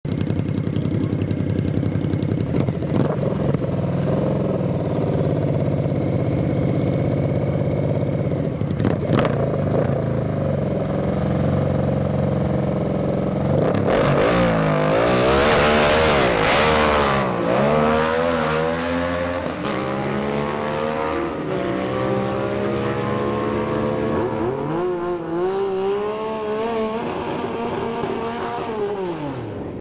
Wellington Invasion - Napa Auto Parts Dragaway - Meremere.
Modified Bike - Qualifying

878  A/MB SUZUKI GSXR1000 998 SUZUKI GEOFF TAUPO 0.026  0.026
74  A/MB 2016 DUCATI 1260CC PAUL MATATA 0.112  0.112